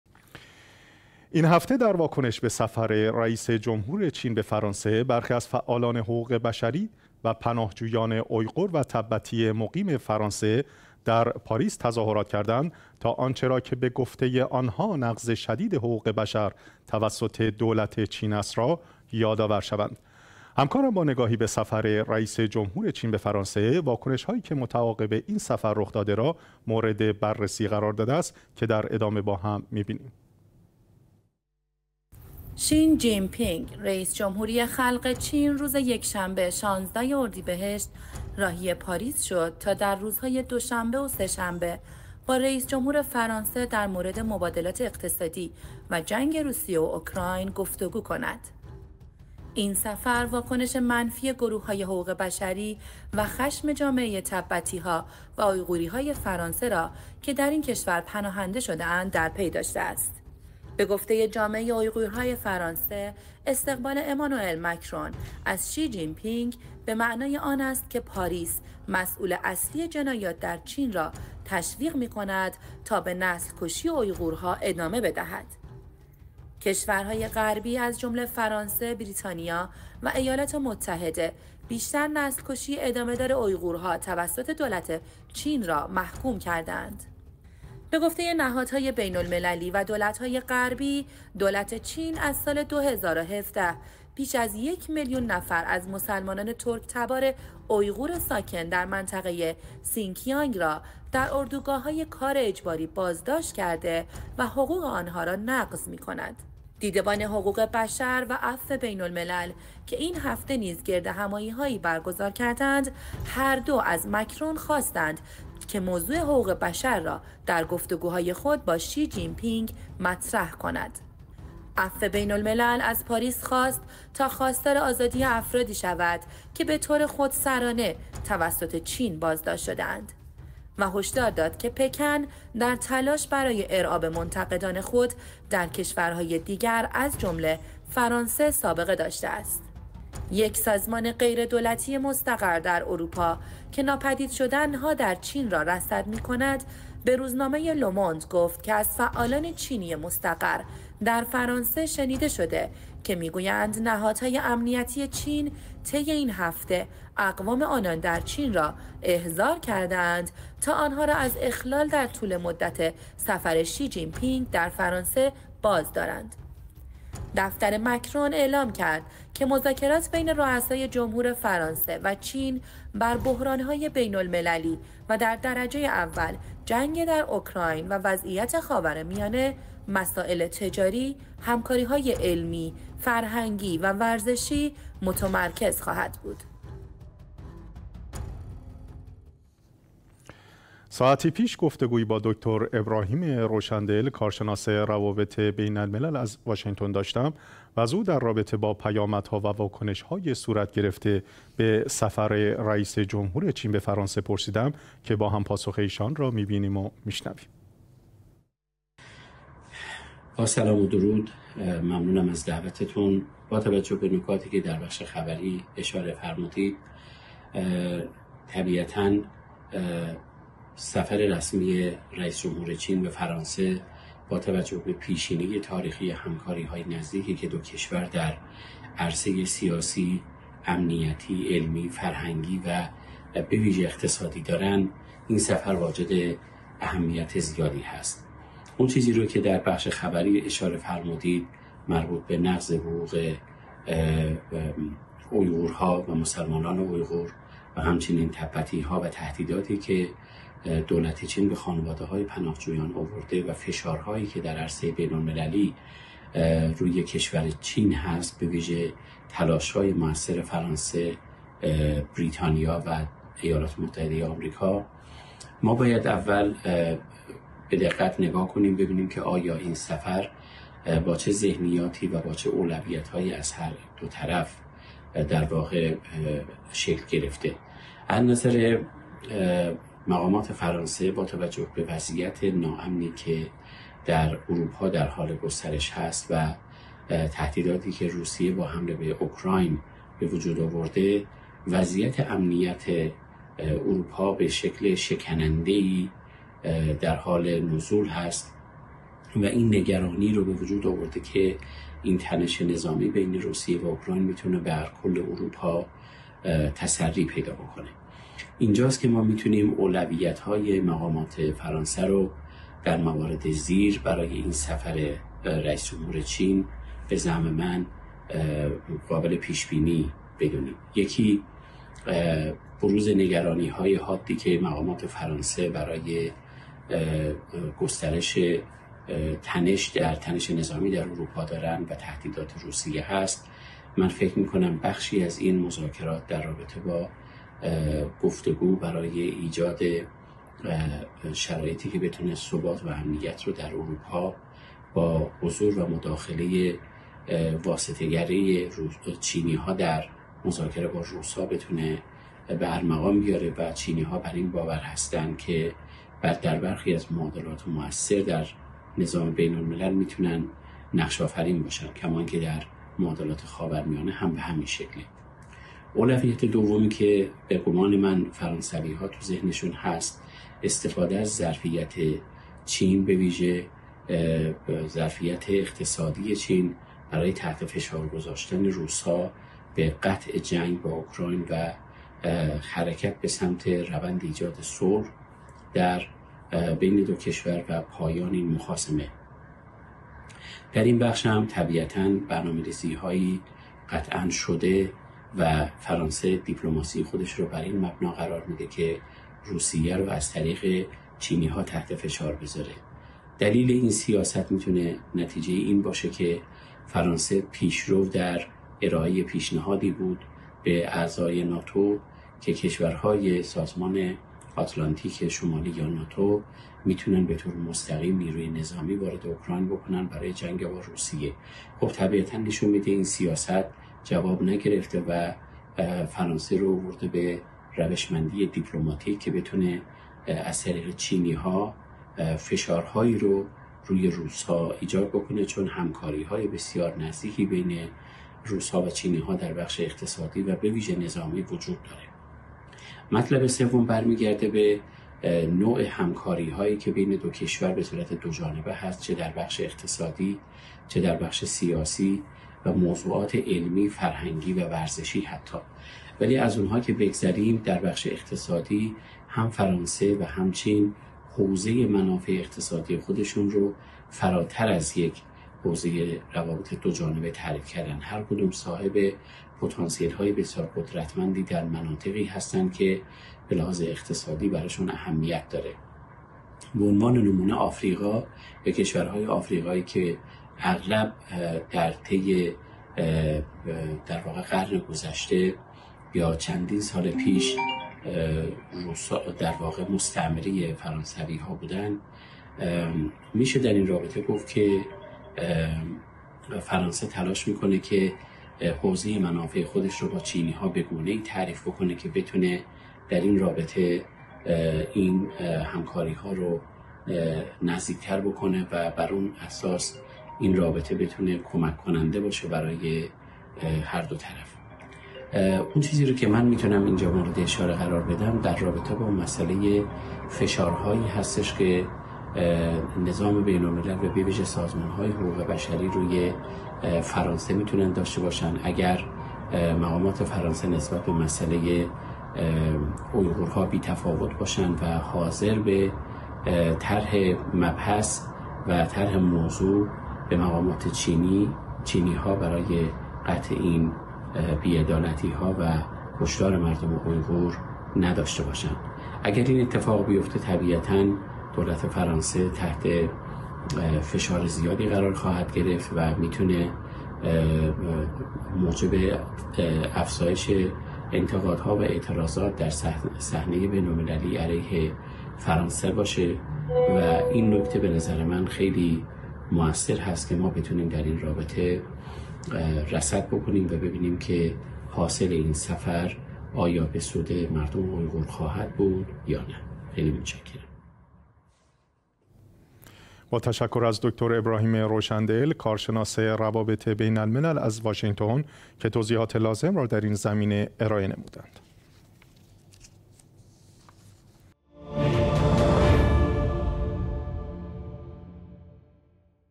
واکنش‌ها به سفر رئیس‌جمهور چین به فرانسه
گفتگوی اخبار شیعه با دکتر ابراهیم روشندل کارشناس روابط بین الملل از واشینگتن 🔸🔸
این هفته در واکنش به سفر رئیس جمهور چین به فرانسه برخی از فعالان حقوق بشری و پناه جویان ایغور و تبتی مقیم فرانسه در پاریس تظاهرات کردند تا آنچه را که به گفته آنها نقض شدید حقوق بشر توسط دولت چین است را یادآور شوند.

شی جین پینگ، رئیس جمهوری خلق چین روز یکشنبه ۱۶ اردیبهشت راهی پاریس شد تا در روزهای دوشنبه و سه‌شنبه با رئیس‌جمهور فرانسه در مورد مبادلات اقتصادی و جنگ روسیه و اوکراین گفت‌وگو کند.

 این سفر، واکنش منفی گروه‌های حقوق بشری و خشم «جامعه تبتی‌ها و اویغورهای فرانسه» را که در این کشور پناهنده شده‌اند، در پی داشته است.

به‌گفته جامعه اویغورهای فرانسه، استقبال امانوئل مکرون از شی جین پینگ، به‌معنای آن است که پاریس «مسئول اصلی جنایات در چین را تشویق می‌کند تا به نسل کُشی اویغورها ادامه بدهد».

کشورهای غربی از جمله فرانسه، بریتانیا و ایالات متحده، پیشتر «نسل کشی ادامه‌دار اویغورها توسط دولت چین» را محکوم کرده‌اند.

به گفته نهادهای بین‌المللی و دولت‌های غربی، دولت چین از سال ۲۰۱۷ بیش از یک میلیون نفر از مسلمانان ترک‌تبار اویغور ساکن در منطقه سینکیانگ را در اردوگاه‌های کار اجباری بازداشت کرده است و حقوق آنها را نقض می‌کند.

دیده‌بان حقوق بشر و عفو بین‌الملل که این هفته نیز گردهمایی‌هایی برگزار کردند، هر دو از ماکرون خواسته‌اند که موضوع حقوق بشر را در گفتگوهای خود با شی جین‌پینگ مطرح کند.

عفو بین‌الملل از پاریس خواست تا خواستار آزادی افرادی شود که به‌طور خودسرانه توسط چین بازداشت شده‌اند و هشدار داد که پکن در تلاش برای ارعاب منتقدان خود در کشورهای دیگر از جمله فرانسه سابقه داشته است.

یک سازمان غیردولتی مستقر در اروپا که ناپدید شدن‌ها در چین را رصد می‌کند، به روزنامه لوموند گفت که از فعالان چینی مستقر در فرانسه شنیده شده است که می‌گویند نهادهای امنیتی چین طی این هفته اقوام آنان در چین را احضار کرده‌اند تا آنها را از اخلال در طول مدت سفر شی جین‌پینگ در فرانسه بازدارند.

دفتر مکرون اعلام کرد که مذاکرات بین روسای جمهور فرانسه و چین بر "بحران‌های بین المللی، و در درجه اول جنگ در اوکراین و وضعیت خاورمیانه، مسائل تجاری، همکاری‌های علمی، فرهنگی و ورزشی" متمرکز خواهد بود.